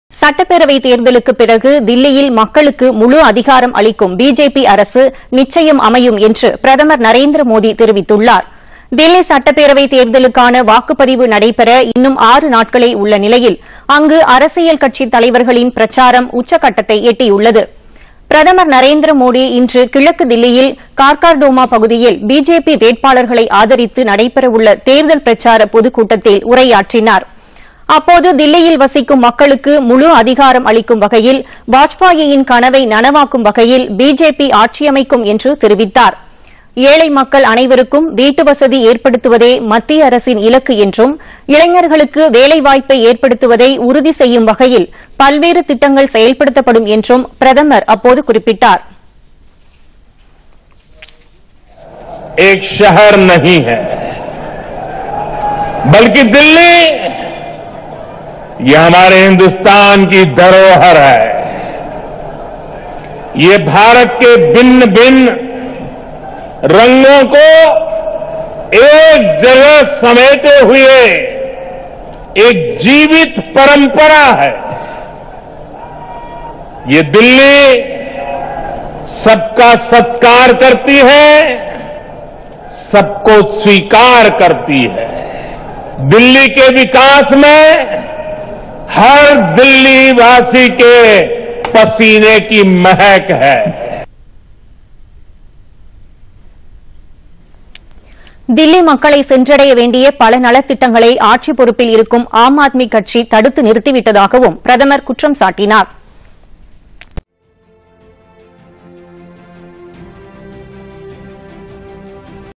பிரதமர் நரேந்திர மோதி நம்பிக்கை
தில்லி சட்டப்பேரவை தேர்தலில் பிஜேபி ஆட்சியைப் பிடிக்கும் - பிரதமர் நரேந்திர மோதி நம்பிக்கை.